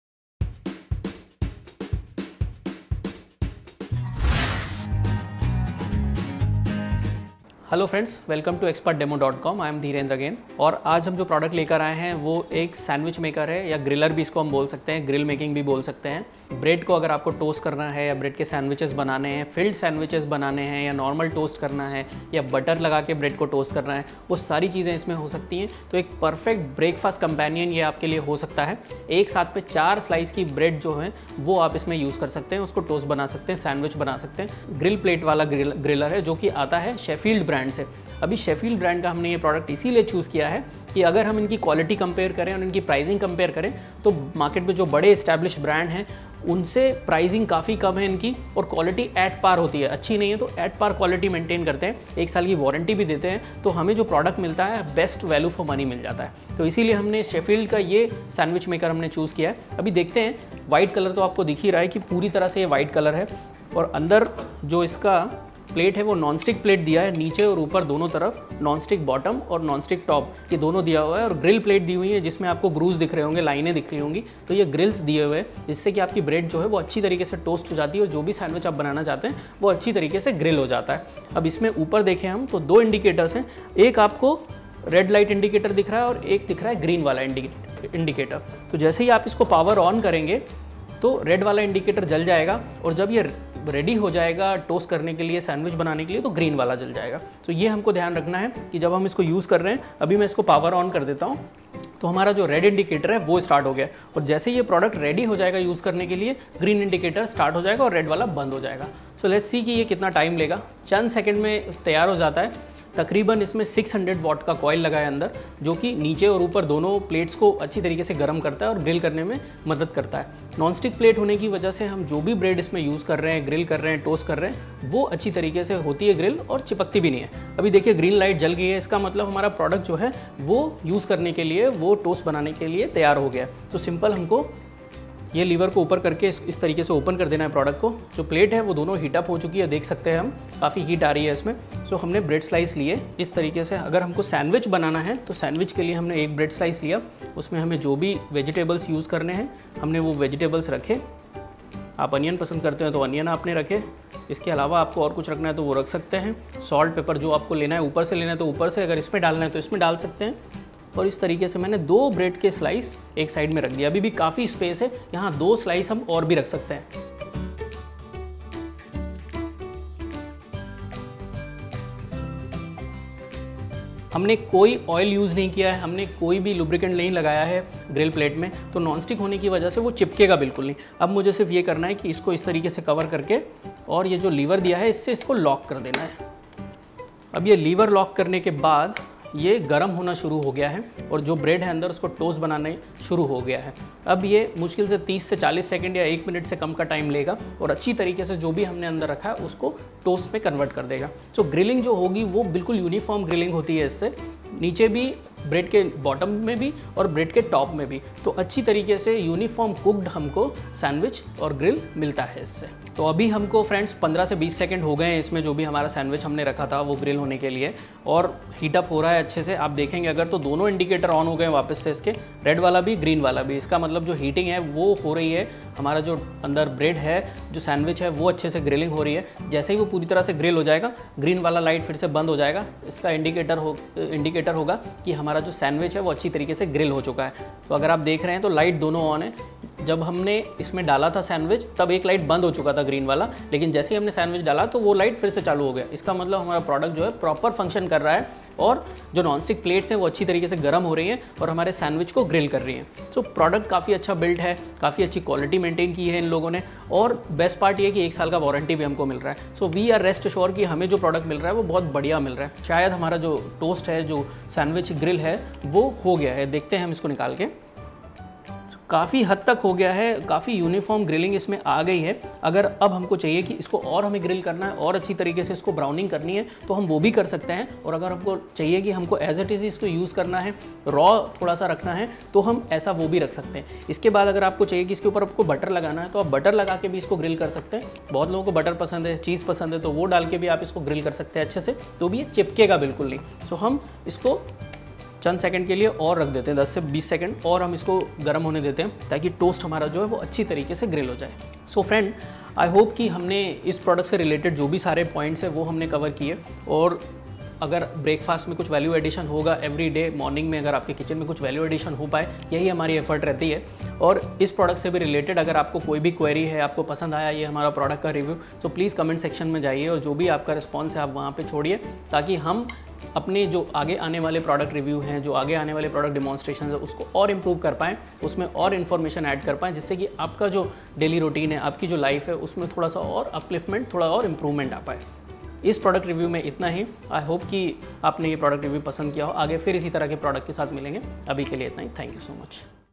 Grill Sandwich Maker for daily breakfast needs and quick snacks
In this video we are demonstrating a perfect budget breakfast companion. This Grill Sandwich Maker will be a great help in everyday breakfast needs.

Market Price: Rs.1499/-
More Toasters